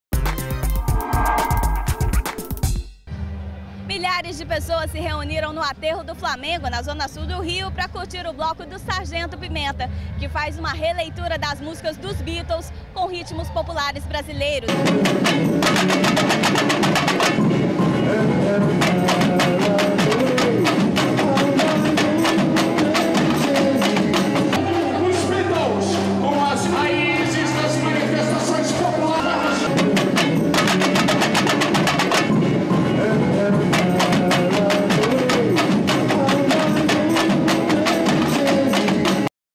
Bloco Sargento Pimenta
Bloco Sargento Pimenta une Beatles e ritmos brasileiros para arrastar foliões no Rio.